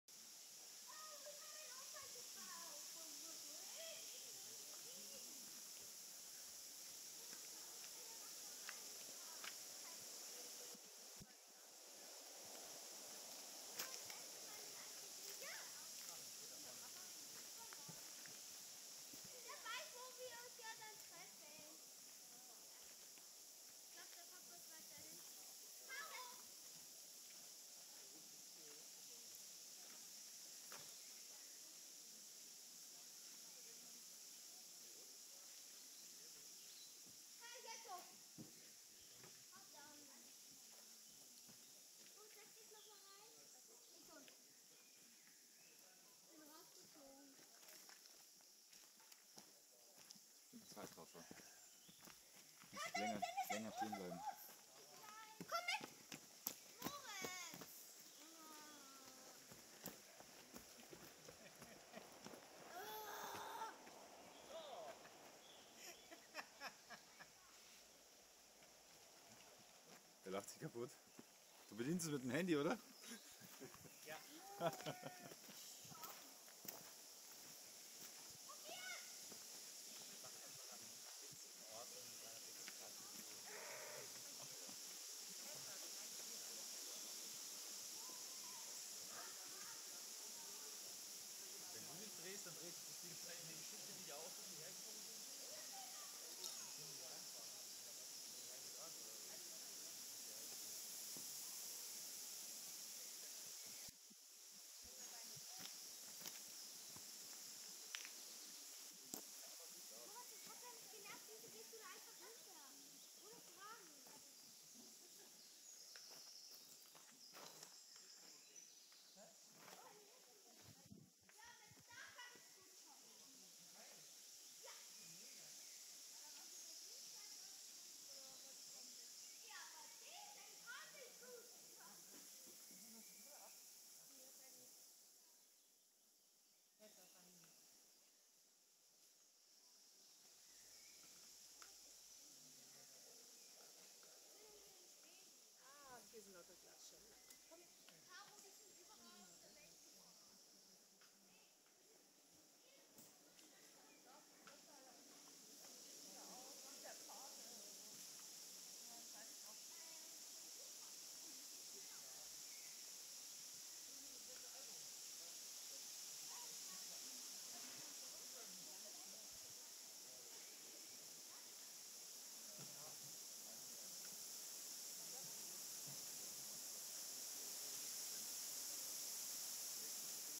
BSC2019 Autofriedhof Ryd 360° Video
Aufenthalt während der Baltic Sea Circle Rally 2019 in Ryd im schwedischen Småland. Die Aufgabe war, den angeblichen Tourbus von Abba zu finden und zu fotografieren.

Grüße an die Chaos City Riders :D